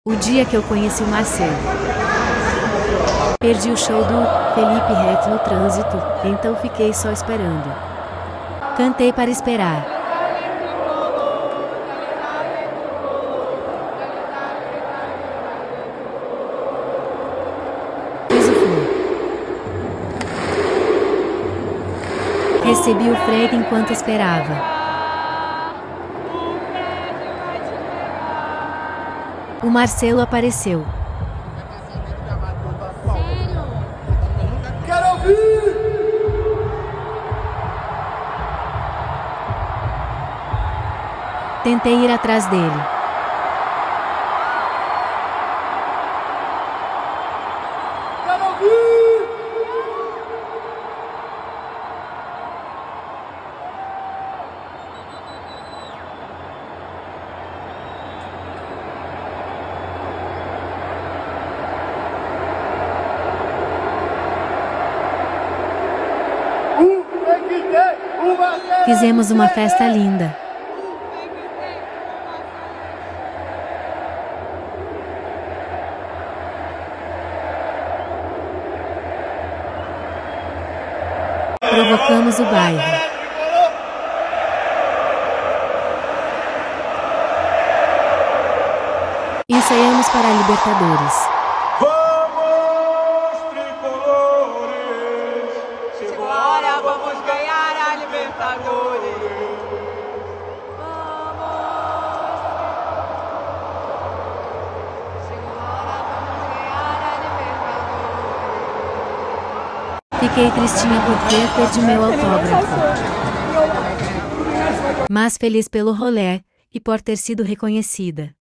APRESENTAÇÃO DO MARCELO | FLUMINENSE
Ontem foi a apresentação do Marcelo no Fluminense e resolvi compartilhar com vocês um pouquinho dessa experiência.

A apresentação foi no Maracanã, teve show do Felipe Ret, porém, eu acabei perdendo o show por conta do trânsito até o estádio. Depois de alguma espera, entraram Fred, Ganso, Cano, Nino, Arias… foi emocionante.

A chagada do Marcelo com a família foi linda, nem consegui acreditar! Além de cantar muito e comemorar, também demos aquela provocada nos rivais.

A única coisa triste foi não ter conseguido um autógrafo, mas fui reconhecida por um seguidor!

Espero que tenham gostado. Saudações Tricolores!